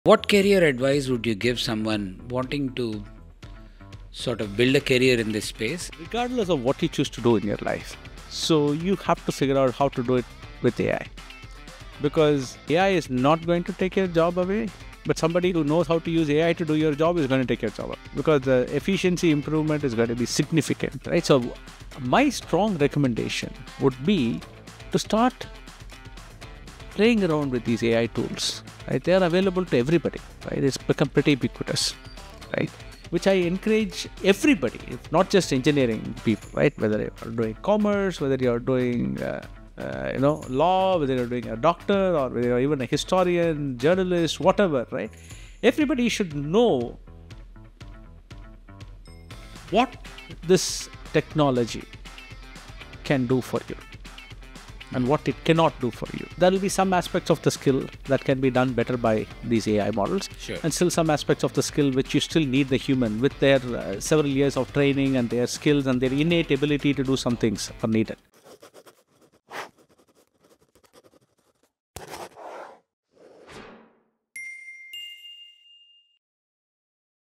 AI in Every Field: Career Advice from Prof. Balaraman Ravindran
Prof. Balaraman Ravindran, Head of the Wadhwani School of Data Science and Artificial Intelligence at IIT Madras offers valuable career advice for anyone looking to thrive in the age of AI. He emphasizes that no matter the profession, understanding how to use AI tools will be crucial to staying competitive. Prof. Ravindran encourages individuals from all fields—whether law, medicine, engineering, or journalism—to explore AI's potential and learn what it can and cannot do. He highlights that AI will enhance certain tasks, but human expertise remains indispensable for many aspects of work.